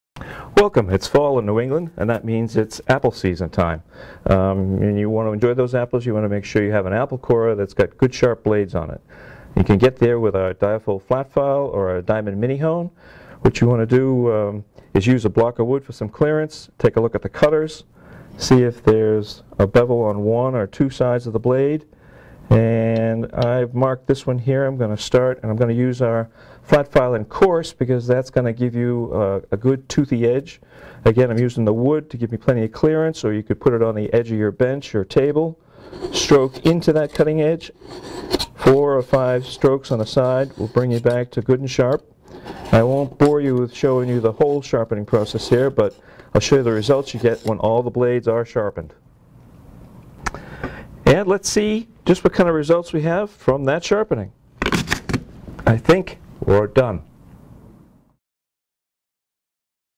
Diafold® Flat File or Mini-Hone® Sharpener for your Apple Corer
Keep your apple corer sharp with DMT!   Make your slicing easier and quicker whether it's just for a few slices or the special apple pie or crisp.